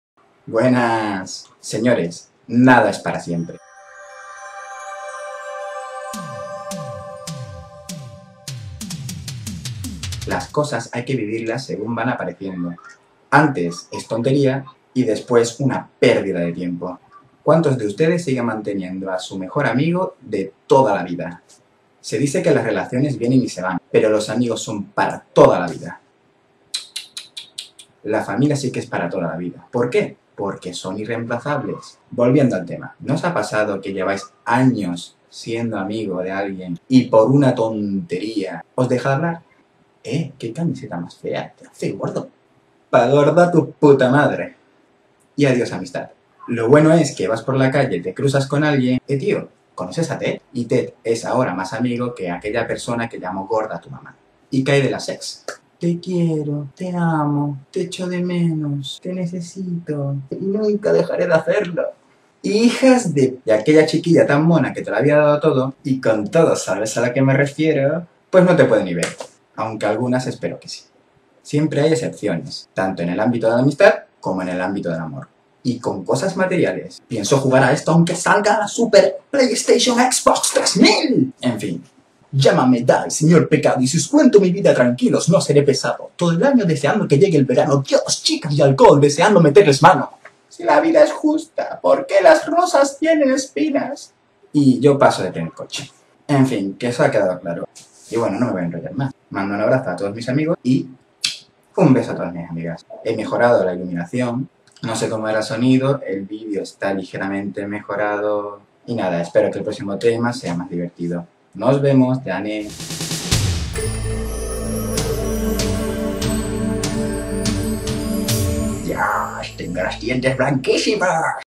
【Vlog #1】 Nada es para siempre
Buahhh, consigo mejorar la calidad del vídeo y la cago terminando cada frase mirando a la derecha (siempre pienso así) T^T Aparte coloqué la vista previa muy abajo... error que no volveré a cometer. Y de nuevo sin peinar y con gafas ¡¡No tengo remedio!!

Si os ha gustado manita arriba, si no, pues manita abajo. Yo con que haya manitas me basta. Y nada... ya haré más, os lo prometo (y sin tantos errores)